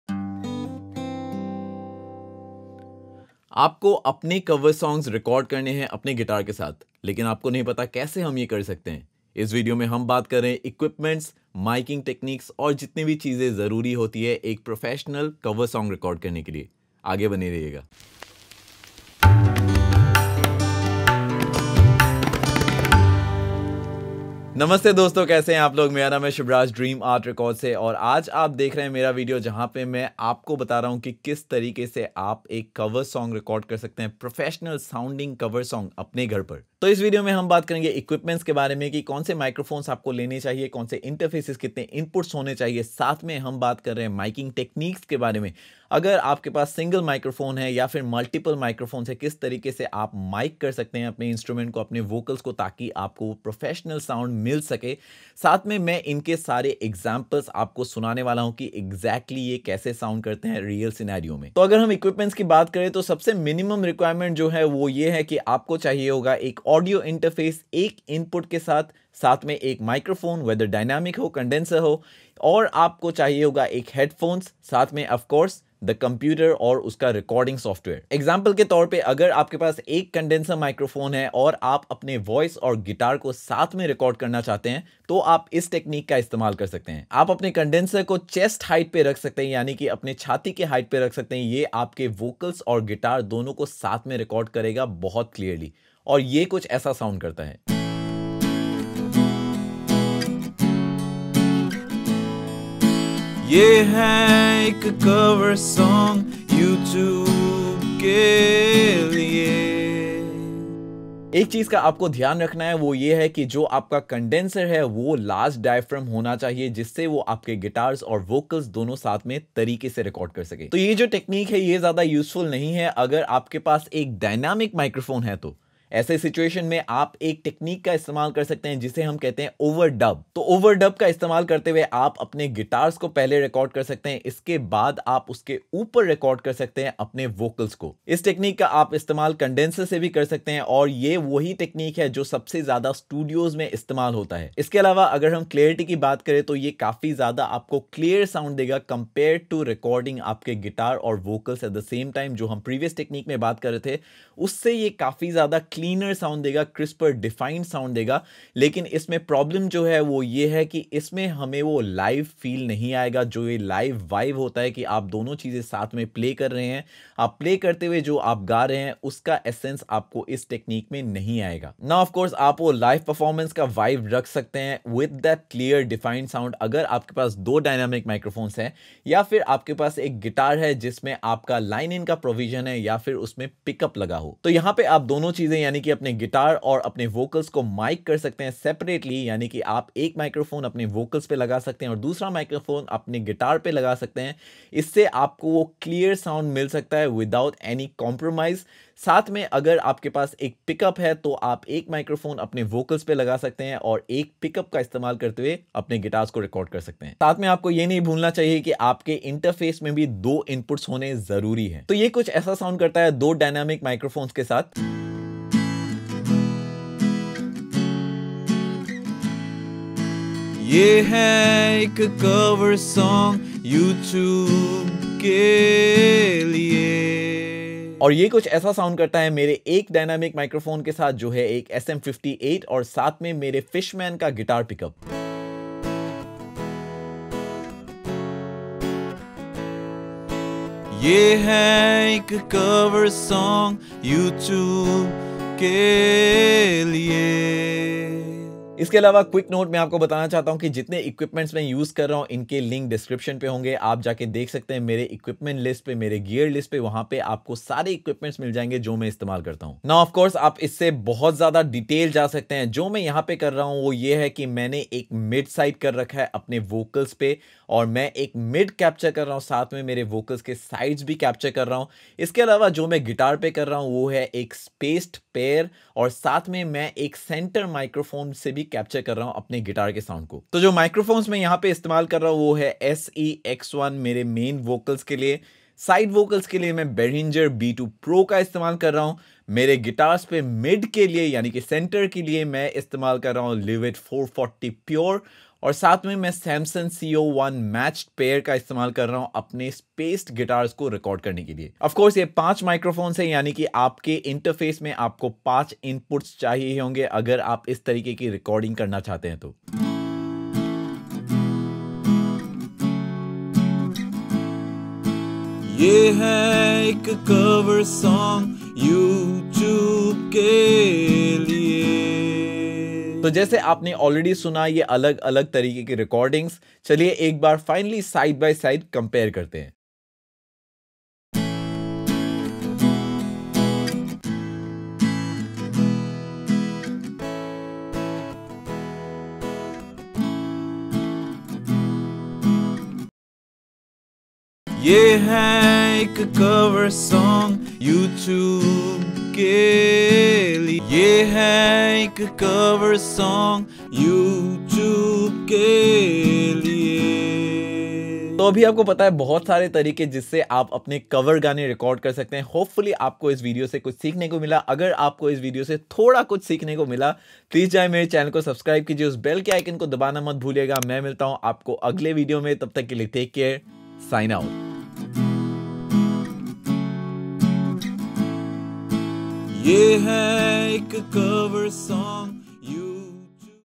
HOW TO RECORD COVER SONGS AT HOME | HINDI | 2021
Hello everyone,
In this video I am showing you, how to record songs at your home with live examples. This video covers topics such as equipments, miking as well as recording techniques involved in recording a professional sounding cover song.

Timestamps-
00:00 - Intro
01:03 - Basic Equipments
01:20 - One Condenser Mic Recording Technique
02:01 - One Dynamic Mic Recording Technique
02:52 - Recording with 2 Mics
03:48 - Recording with 1 Mic & Guitar Pickup
04:20 - Recording with 5 Mics ( Master Technique )
05:32 - Final side by side comparisons